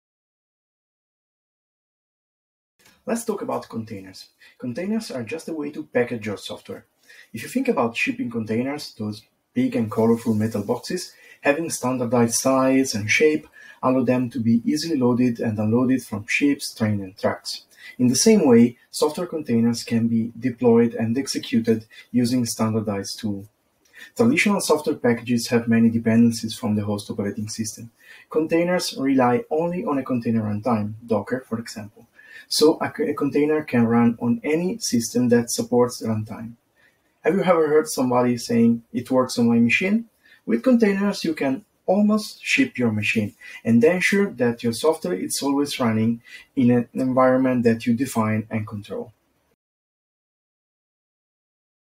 What are Containers?
Learn about Containers in under a minute, as narrated by our subject-matter expert, Valter Minute!

This short video is part of the Toradex Know-Your-Stuff series.